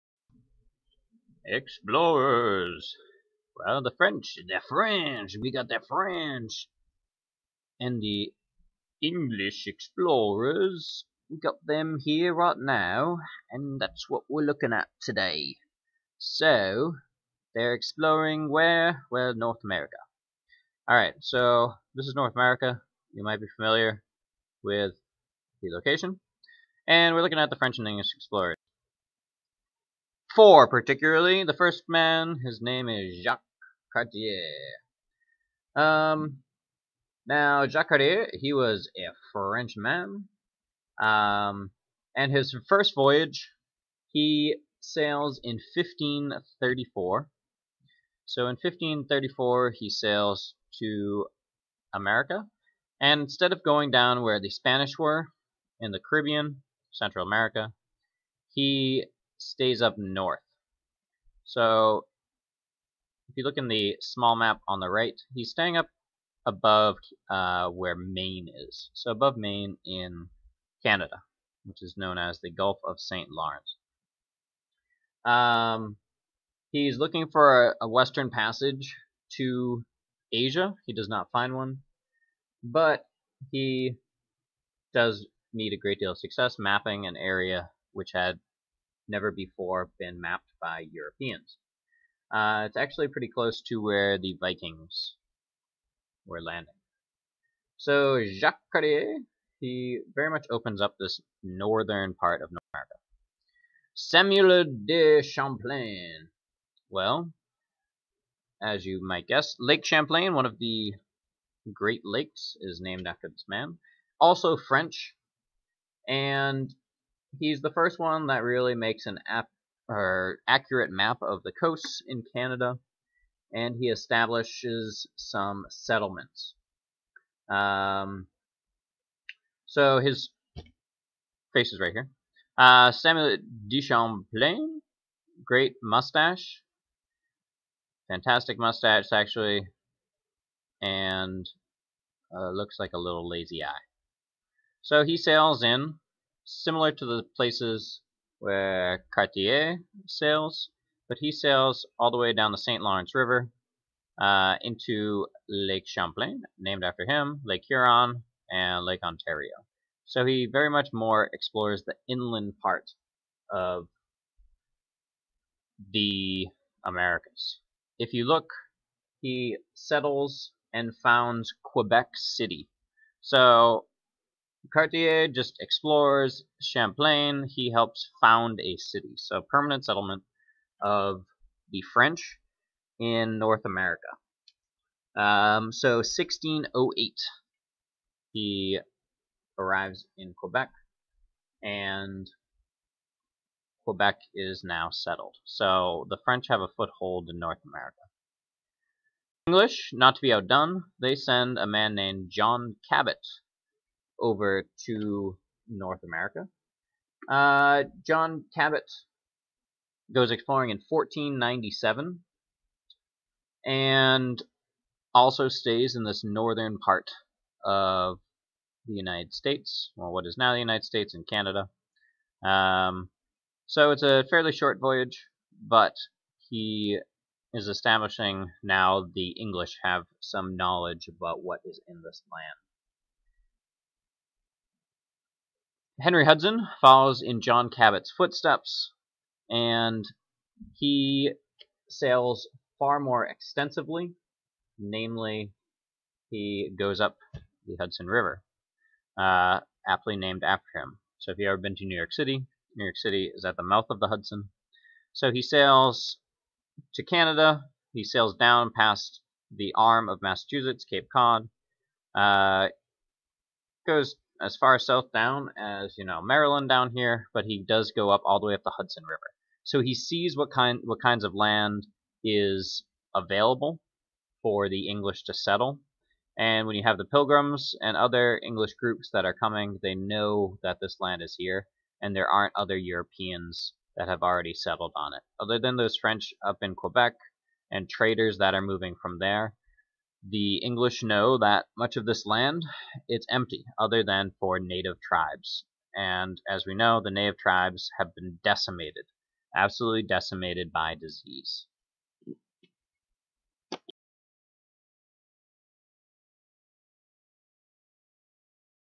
French and English Explorers
People: WHERE DID THEY EXPLORE???
1. Jacques Cartier    2. Samuel de Champlain    3. John Cabot    4. Henry Hudson